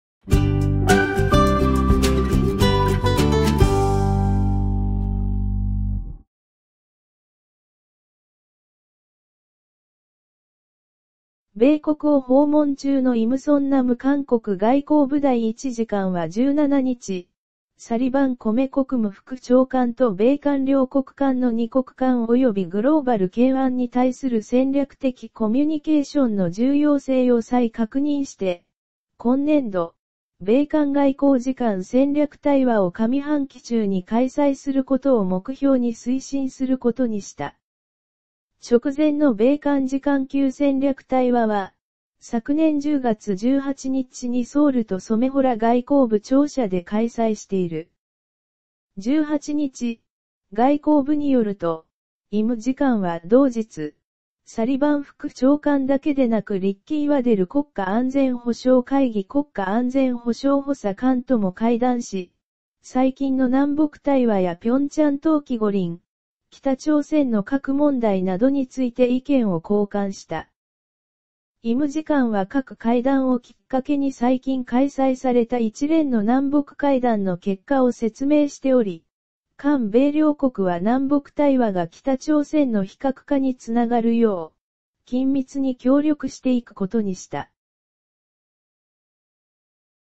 韓米外務次官、ことし上半期中の次官級戦略対話を推進 (1/18)
韓米外務次官、ことし上半期中の次官級戦略対話を推進

米国を訪問中のイム・ソンナム韓国外交部第1次官は17日（現地時間）、サリバン米国務副長官と米韓両国間の二国間およびグローバル懸案に対する戦略的コミュニケーションの重要性を再確認して、今年度、米韓外交次官戦略対話を上半期中に開催することを目標に推進することにした。

　直前の米韓次官級戦略対話は、昨年10月18日にソウル・都染洞（トリョムドン）外交部庁舎で開催している。

　18日、外交部によると、イム次官は同日、サリバン副長官だけでなくリッキー・ワデル国家安全保障会議（NSC）国家安全保障補佐官とも会談し、最近の南北対話や平昌冬季五輪、北朝鮮の核問題などについて意見を交換した。

　イム次官は各会談をきっかけに最近開催された一連の南北会談の結果を説明しており、韓米両国は南北対話が北朝鮮の非核化につながるよう、緊密に協力していくことにした。